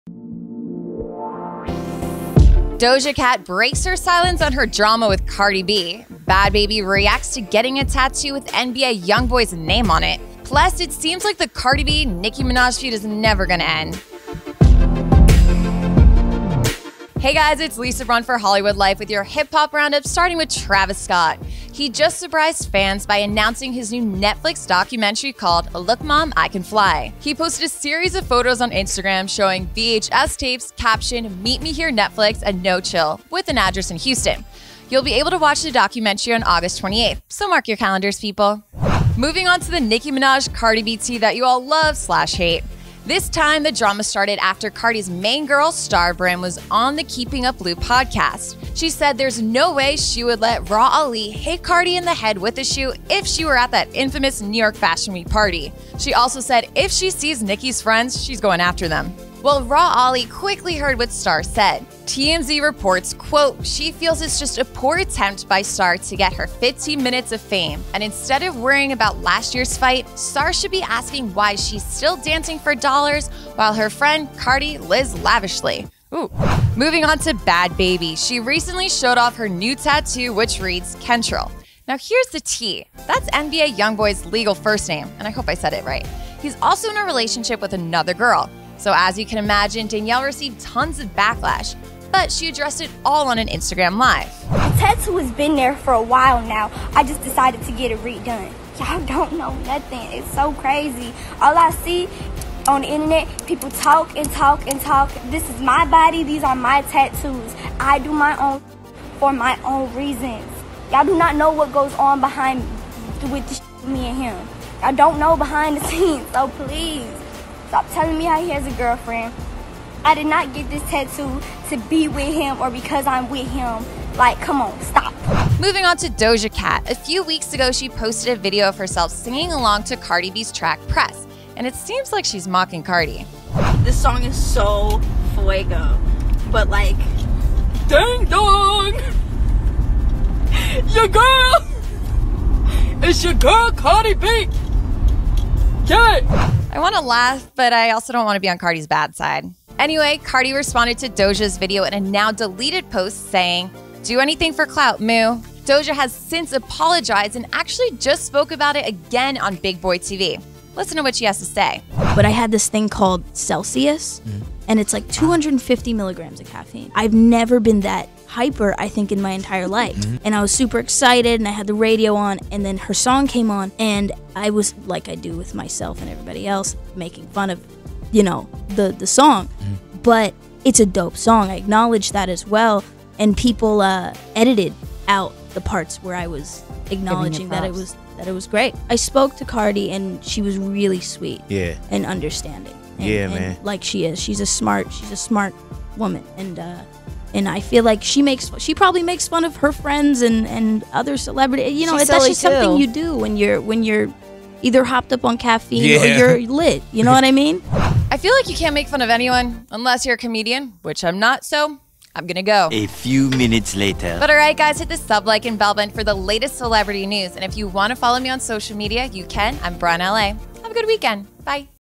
Doja Cat Breaks Silence On Cardi B Drama In New Video
Doja Cat breaks her silence on her drama with Cardi B. Bhad Bhabie reacts to getting a tattoo with NBA Young Boy’s name on it. Plus – it seems like the Cardi B/Nicki Minaj feud is never going to end.

Cardi B, 26, spent some mother-daughter time with her hilarious one-year-old daughter Kulture on Aug. 17 and she captured a moment to remember all on film. The rapper appeared to be in a department store near a rack of clothing with her precious tot in a video she shared to Instagram. In the clip, little Kulture can be seen up-close before giving her mom some sassy side eye as she puts the camera on her and touches her arm to get and try her to look. Cardi then looks into the camera with a funny look of disbelief at her baby girl’s reaction. “😒🙄,” she perfectly captioned the post.

Hosting, Producing Lisa Braun @BraunLA

ABOUT HOLLYWOODLIFE